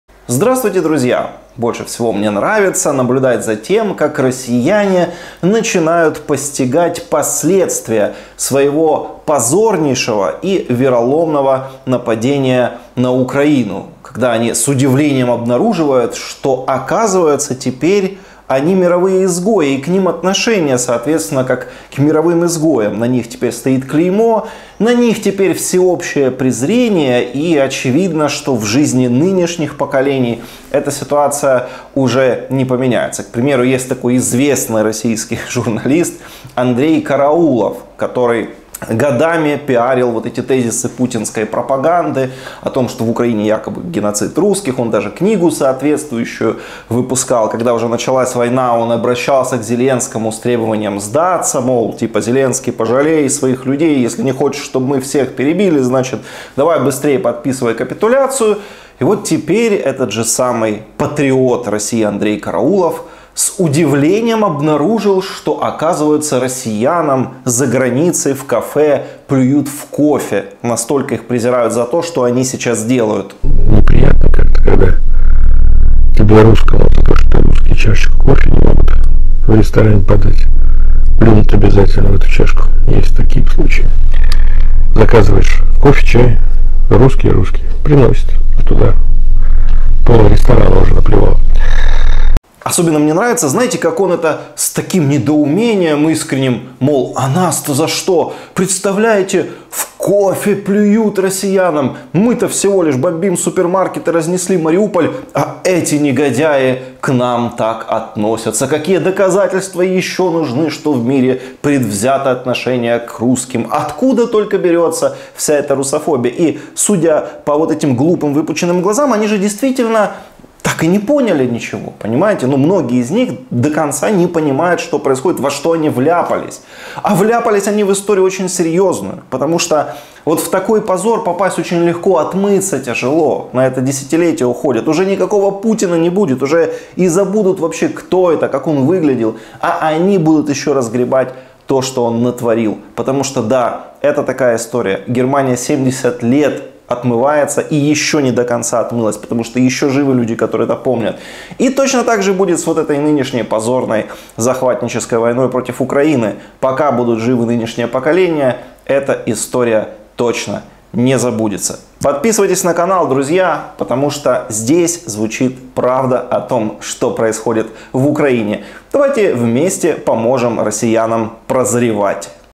"Нам плюют в кофе!" КАРАУЛОВ рассказал об отношении к россиянам в Европе
Поддержите информационную борьбу с агрессором:
💳 Монобанк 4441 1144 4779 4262